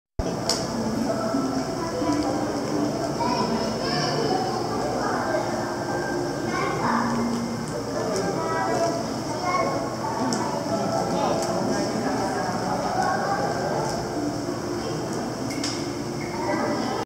トゲチョウチョウウオ、タテジマキンチャクダイ、ハタタテダイ、コガネキュウセンなど 竹島水族館 愛知県蒲郡市 2020年8月5日
COVID-19の第２波の最中なので、休日は避け、平日に訪れました。
夏休み期間中とはいえ、いつもよりはきっと入館者は少ないのでしょう。
混雑している状態ではありませんでした。
もちろんマスクをして入館しました。
館内はすべてフラッシュ撮影もOKとなっています。

なかなかにぎやかで華やかな水槽です。